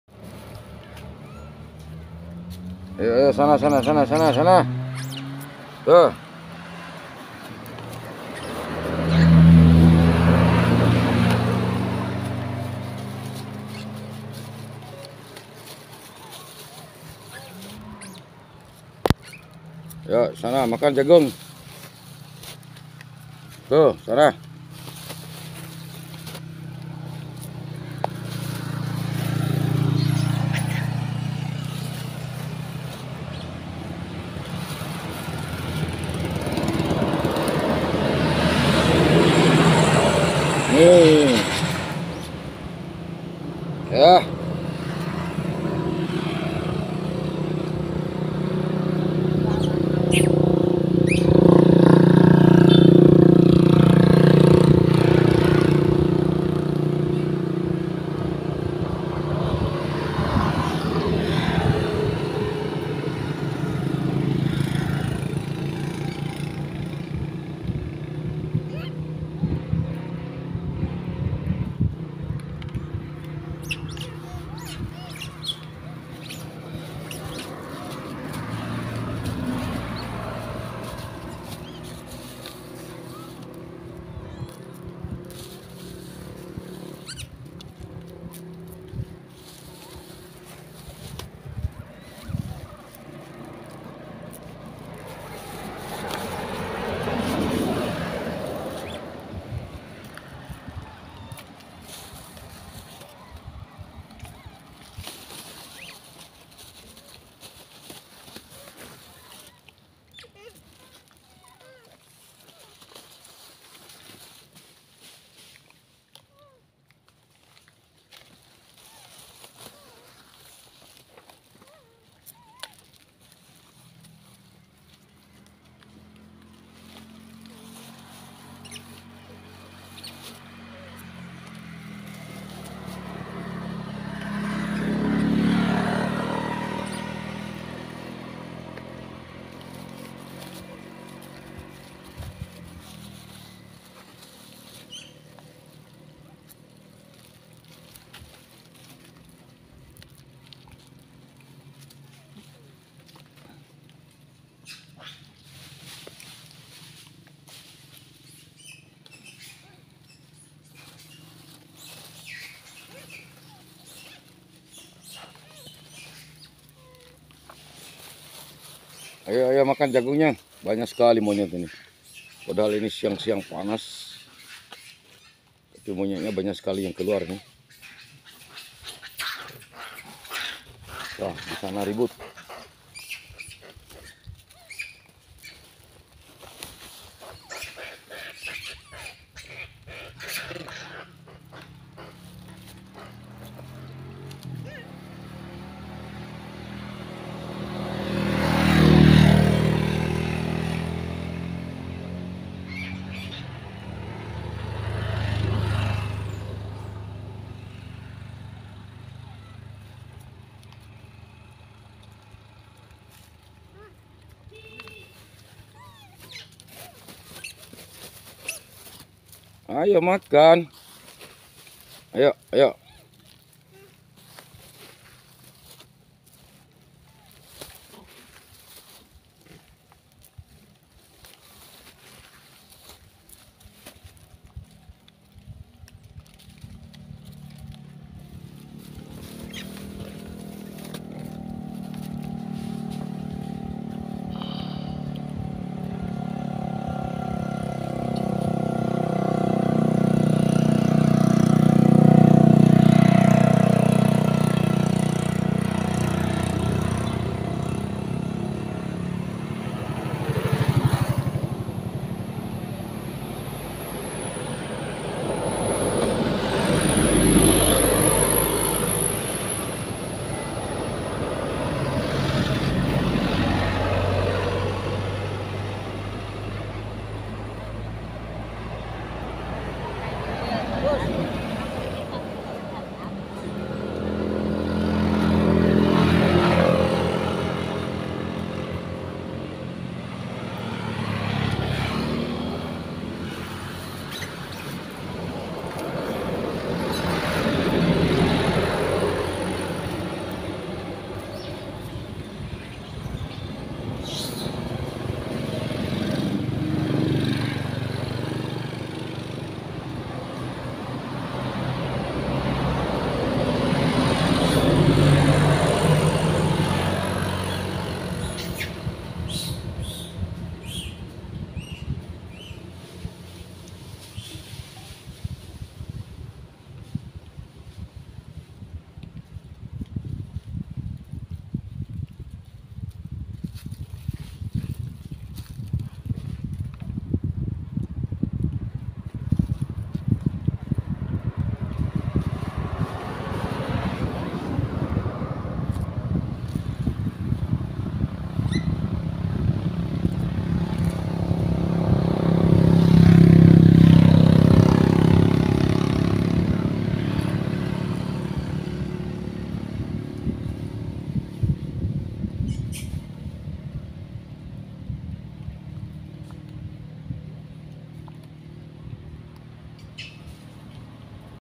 panasnya siang ini dapat lihat ribuan monyet
ribuan monyet di bawakan makan jagung siang ini @BurhanBurhan-xe7vi